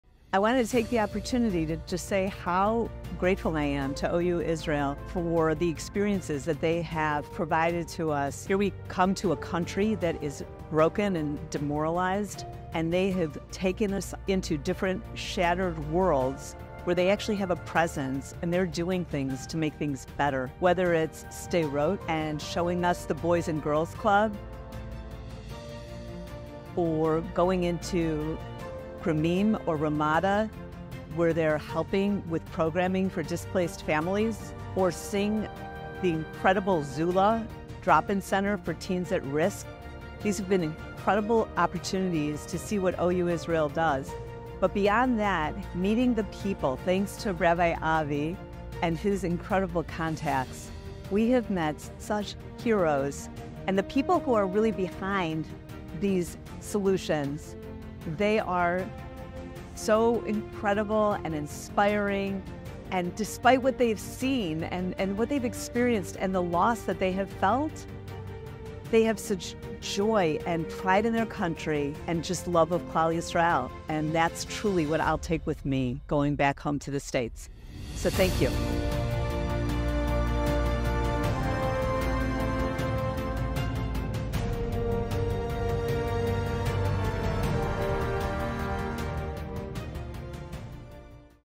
WHAT DOES ISRAEL AND ITS PEOPLE REALLY NEED?
Lisa Baron, St Louis Community Leader.
Joined our OU Israel Woman's Mission.
Here are some highlights from what OU Israel is doing on the ground in Israel.